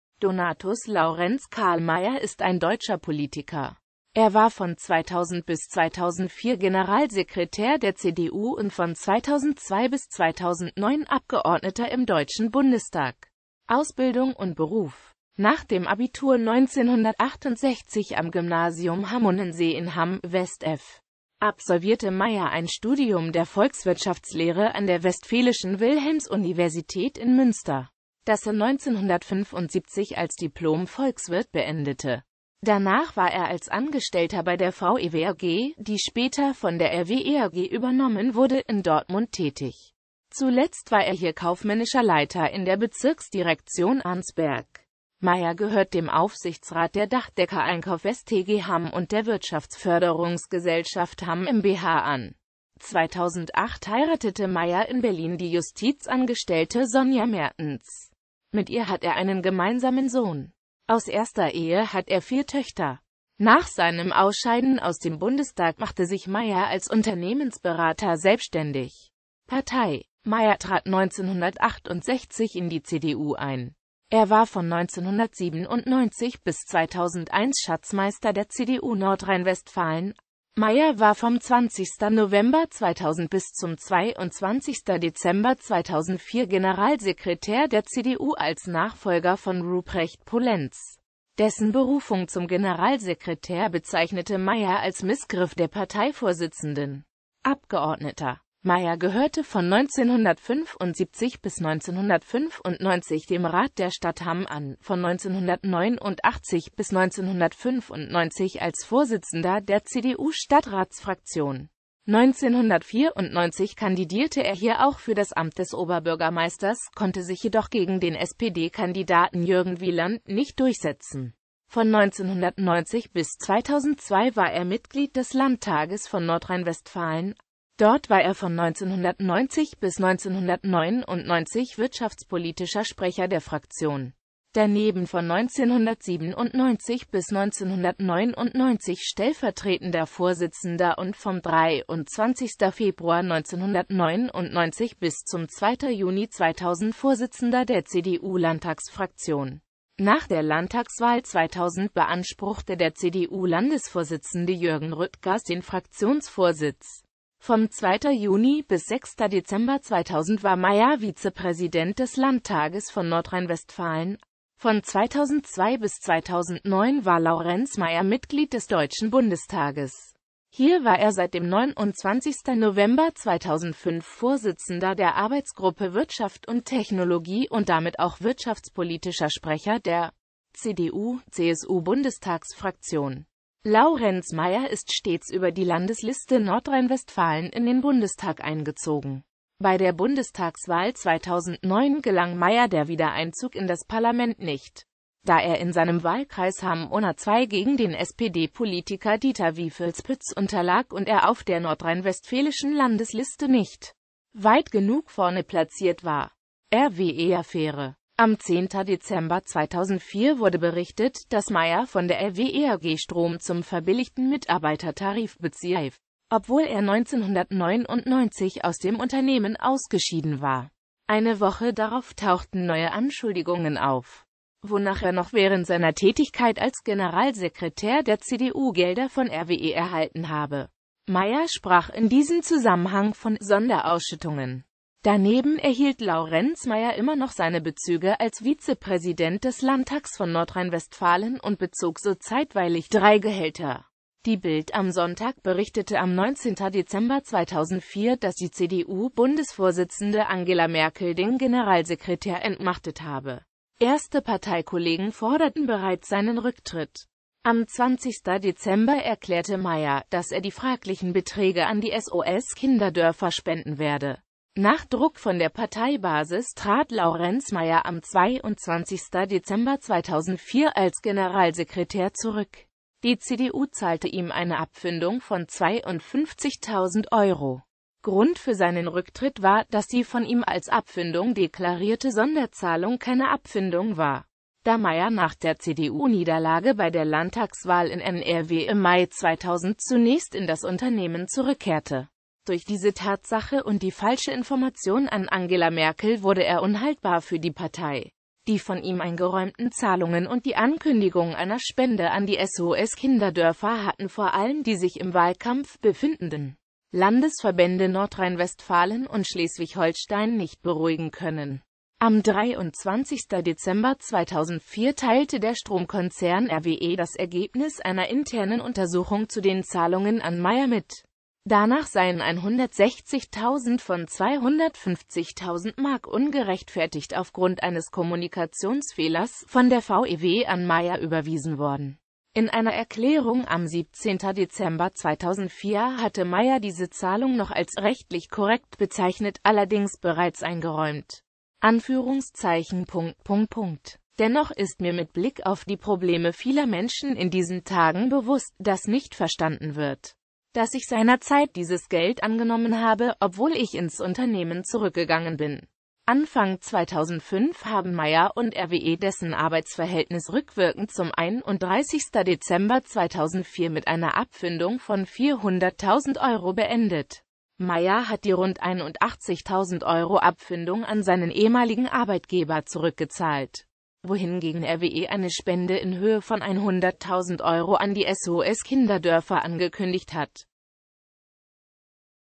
Laurenz Meyer
Vielen Dank für Ihre Unterstützung:

Donatus Laurenz Karl Meyer (* 15.Februar 1948 in Salzkotten) ist ein deutscher Politiker (CDU).Er war von 2000 bis 2004 Generalsekretär der CDU und von 2002 bis 2009 Abgeordneter im Deutschen Bundestag.

Bild-Copyright-Informationen
Urheber Info: Dirk Vorderstraße